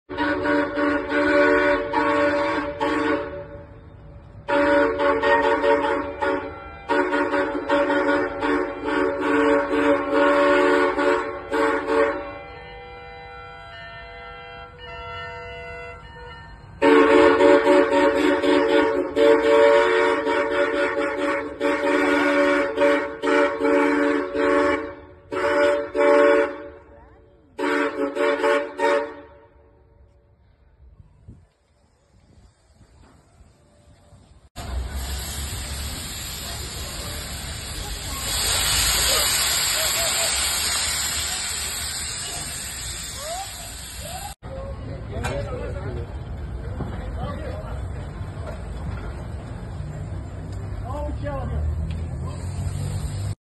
Locomotiva 310 de volta aos trilhos!
A Expoturismo em União da Vitória, que acontece neste final de semana, conta com uma convidada especial: a Maria Fumaça de volta aos trilhos.

Vídeo: recebido pelo whatsapp